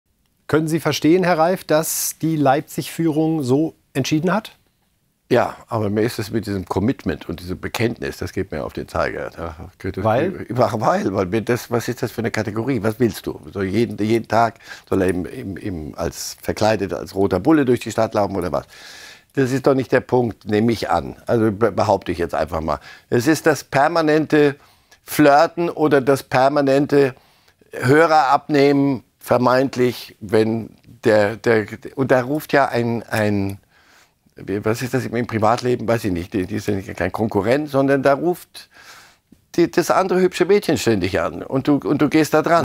Reif genervt von Leipzig-Aussagen: „Soll er als Bulle verkleidet durch die Stadt?“
RB Leipzig hat sich von Max Eberl getrennt. Immer wieder betont der Klub, die Identifikation von Eberl habe gefehlt. Das nervt TV-Legende Marcel Reif etwas.